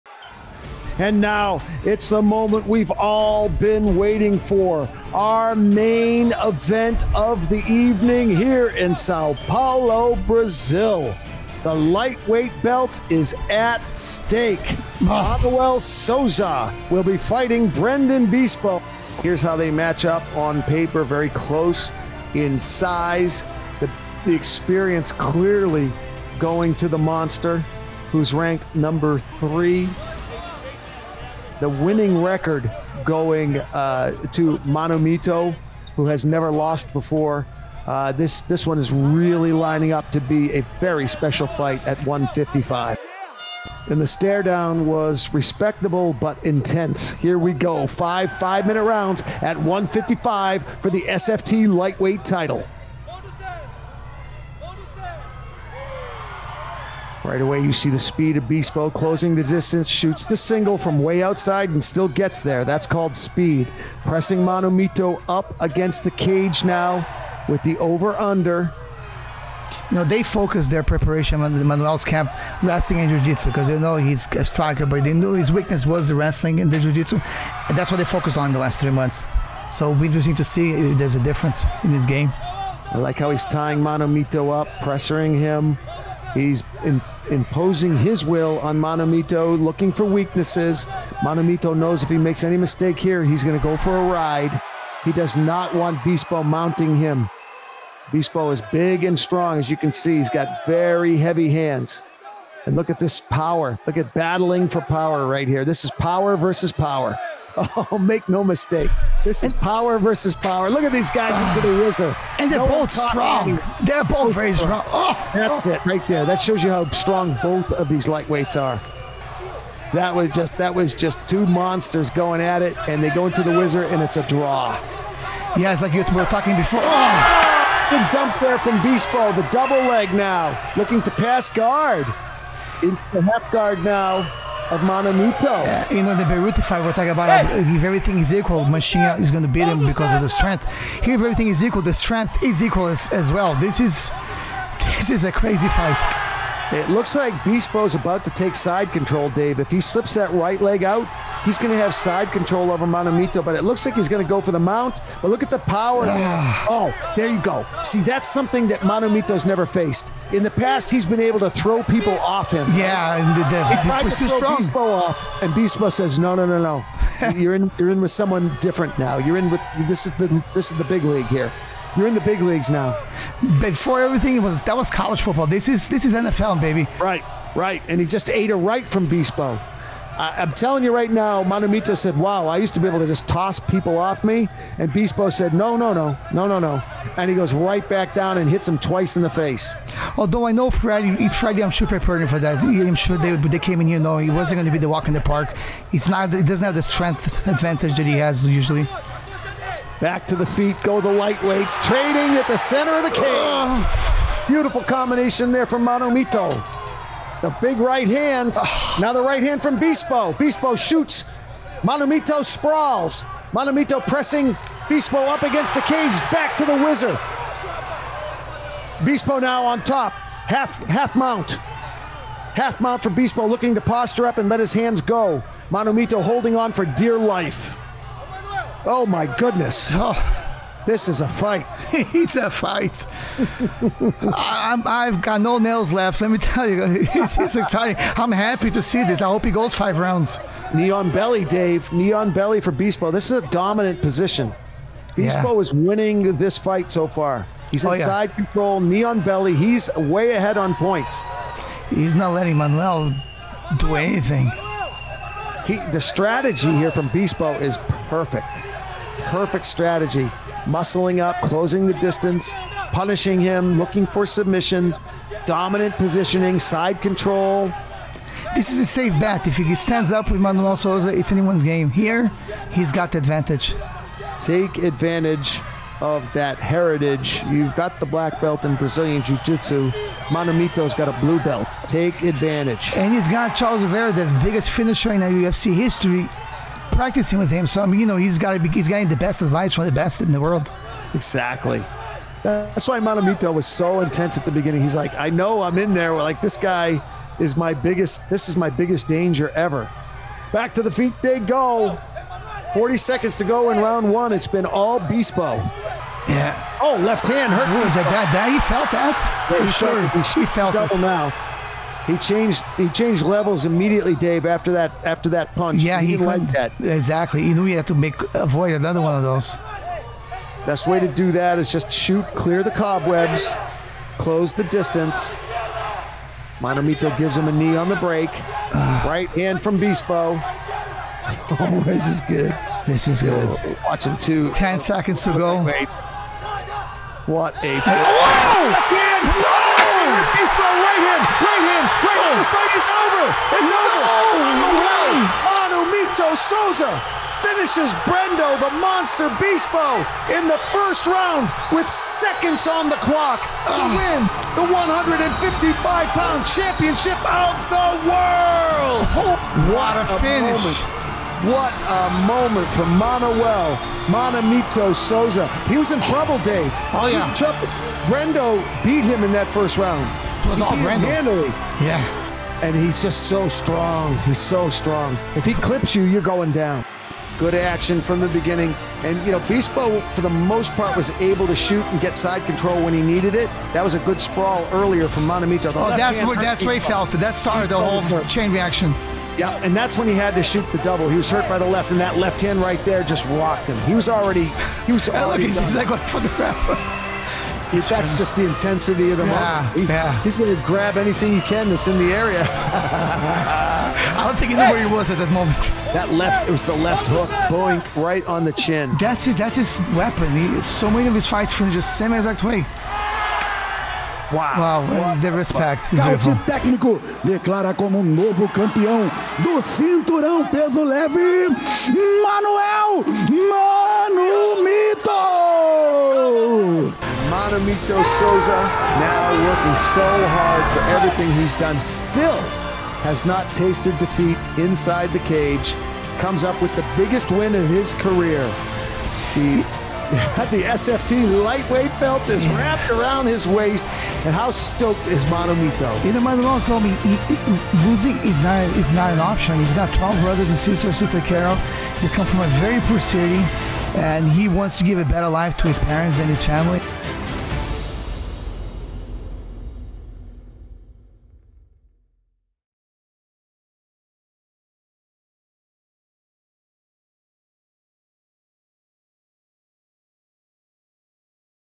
FULL FIGHT MMA | SFT 27 Sousa vs. Bispo - lightweight title fight.
MMA FULL FIGHT - SFT 27's main event  pits SFT’s 2020 Fighter of the Year, Manuel “Maumito” Sousa (6-0), against veteran Brendo “The Monster” Bispo (17-4- for the vacant SFT Lightweight Championship.

The 23-year-old Sousa is a tremendous striker whose overall combat sports record includes 23 straight victories in kickboxing, 18 by knockout, and all six of his pro MMA fights have been victories in SFT competition, none going into the third round.

Currently on a 6-fight win streak, Bispo accepted the challenge of fighting hot prospect Sousa after a few other fighters passed on the opportunity.  Acclaimed as one of the top lightweights in Brazil, Bispo now has the chance to prove he’s No. 1 versus Sousa.  Bispo trains at the famed Chute Boxeo, under the direction of the famed Charles Oliveira.  A solid all-around fighter, the more experienced Bispo has seven victories by knockout, along with five more by submission.

0:00  Intro
0:41 Round 1
6:05 Replay
6:57 Oficial results